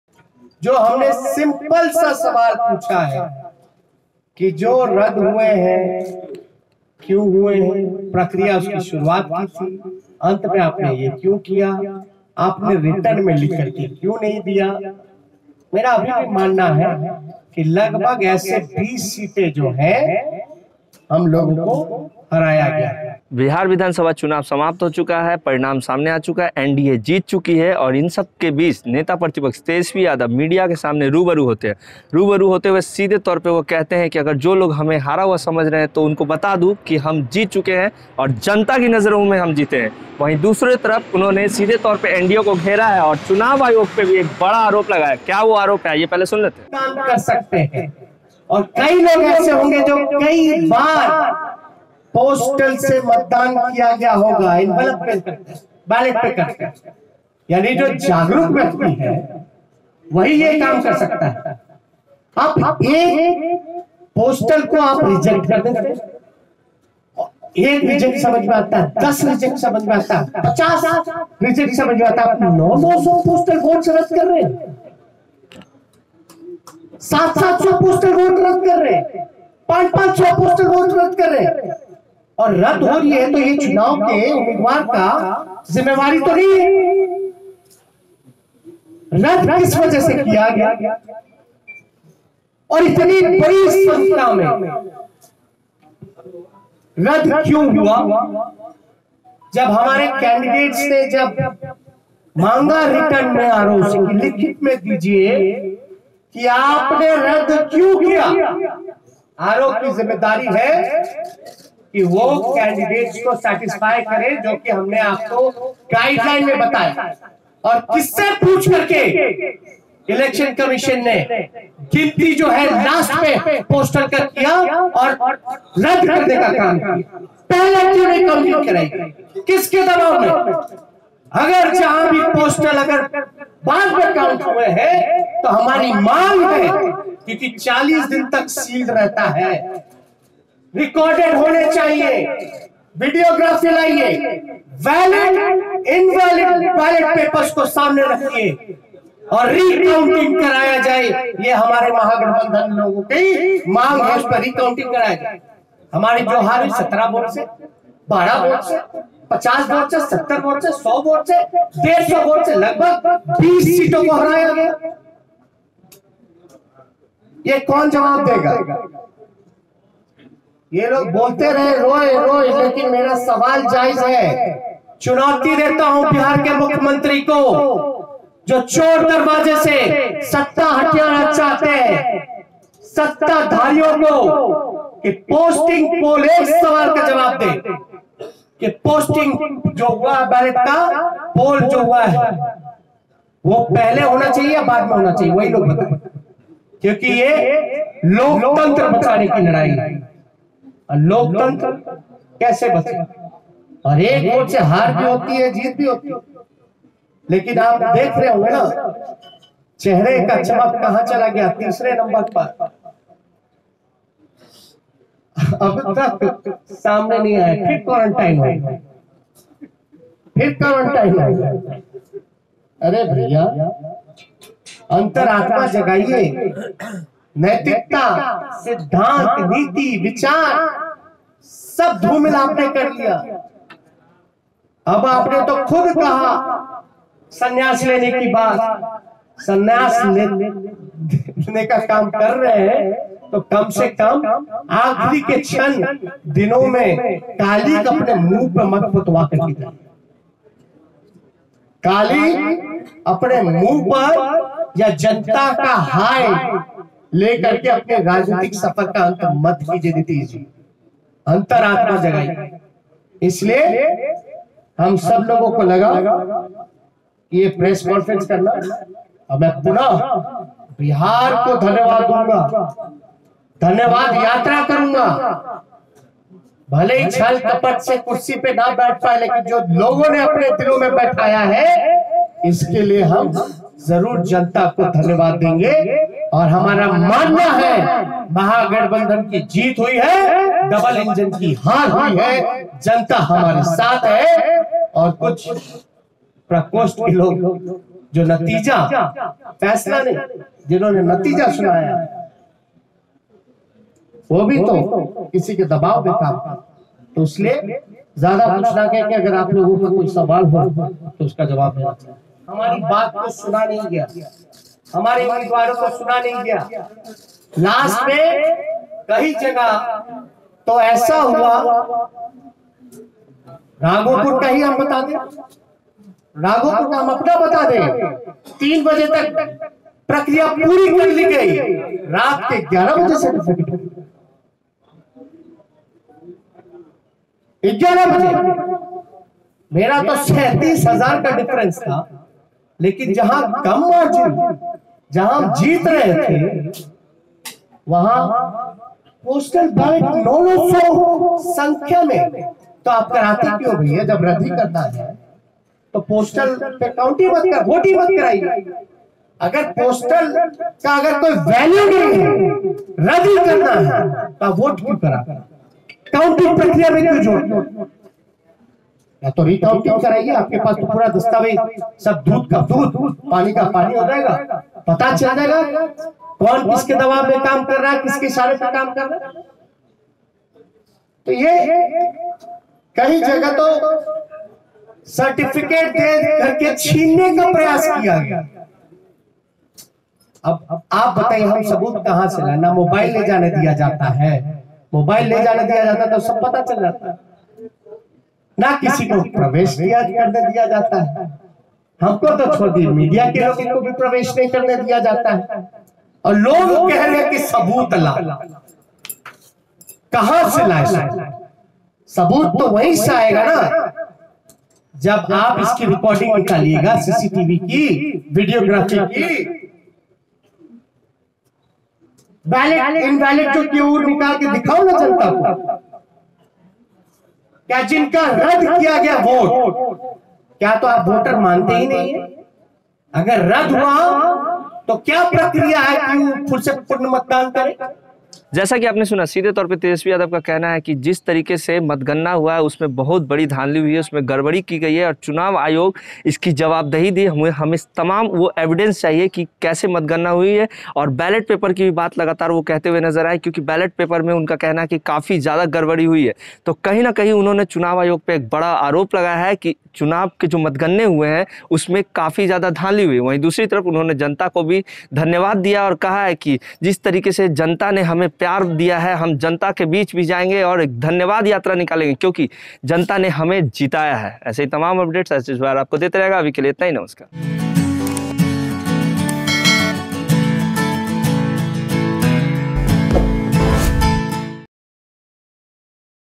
Bihar election result के बाद पहली बार सामने आए Tejashwi| HS BIHAR
Bihar election result के बाद पहली बार सामने आए Tejashwi, Election commission पर लगाया Counting मे छेड़छाड़ करने का आरोप |HS BIHAR